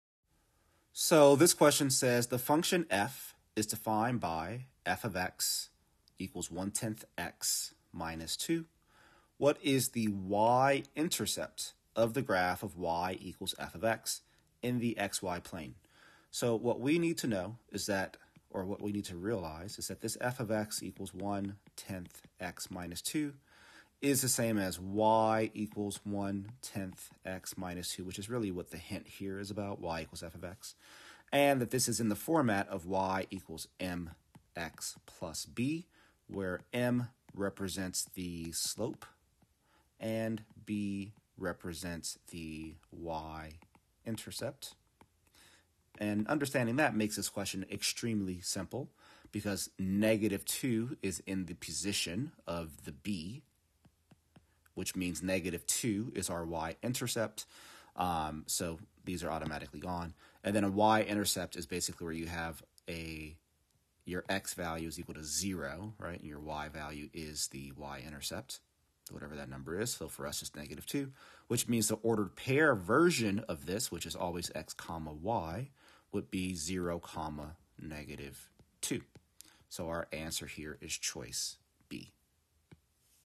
The function f is defined by f(x) = 1/10 x - 2. Whatis the y-intercept of the graph of y = f(x)...
Nonadaptive Digital SAT Practice Test 1 Module 2 Question 8:

The function f is defined by F(x) = 1/10 x - 2. What is the y-intercept of the graph of y = f(x) in the xy-plane?